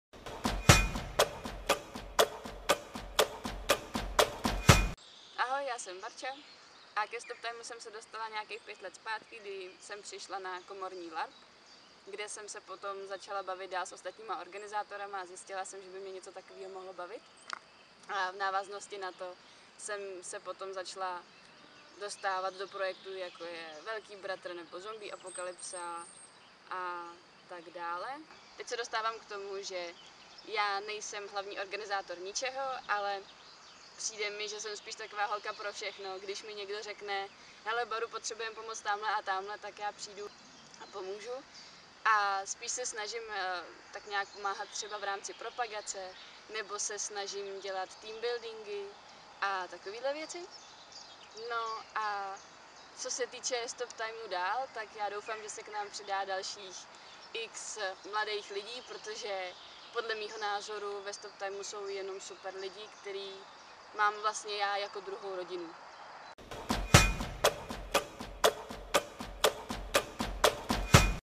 StopTIme 10 let - Barča Sobotková
Barča je ta, díky které dneska ještě funguje StopTime. Jestli máte rádi naše akce a uvědomujete si, že to děláme zadarmo a investujeme do toho hodně času, protože nás to prostě baví, tak hlavně poděkujte této spasitelce. Barča často odmaká kvanta právě těch méně zábavných věcí jako je administrativa, výroba předmětů, domlouvání všeho druhu,... prostě bez ní bychom byli někde jinde.

Barča se stala pravou i levou rukou StopTimu a za její zodpovědnost, pracovitost i vynalézavost má naše uznání a obdiv.

Děkujeme Barunko, jsi zlatíčko!